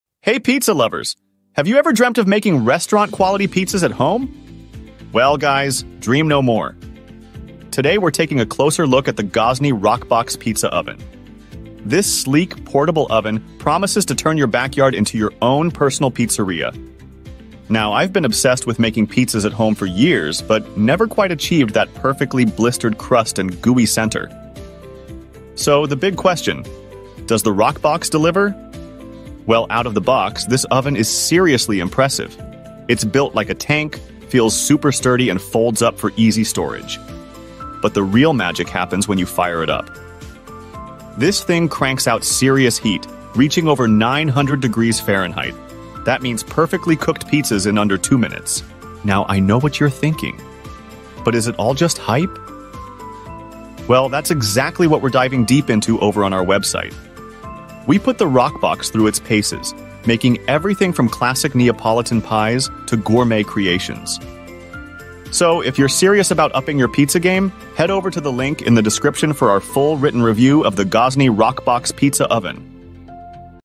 Gozney Roccbox Pizza Oven Review
The Gozney Roccbox emerges as the ultimate portable pizza oven, elevating any outdoor kitchen to new heights. To learn more, check out the full written review